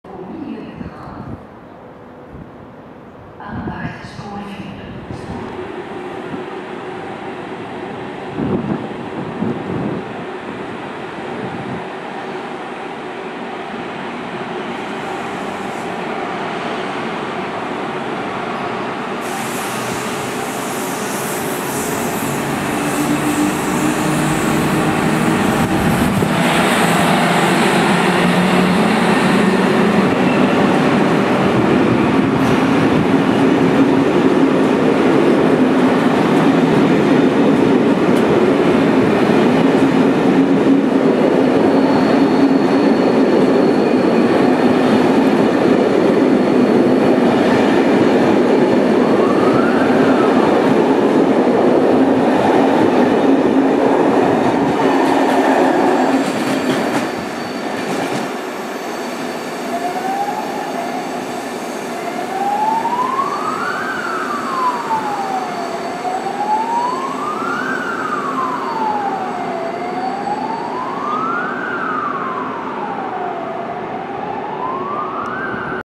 HLE 2138 + M4 vertrekt uit Brussel-Noord
De 2138 met P8906 naar Oudenaarde.
La 2138 avec P8906 vers Audenaarde.
The 2138 with P8906 towards Oudenaarde.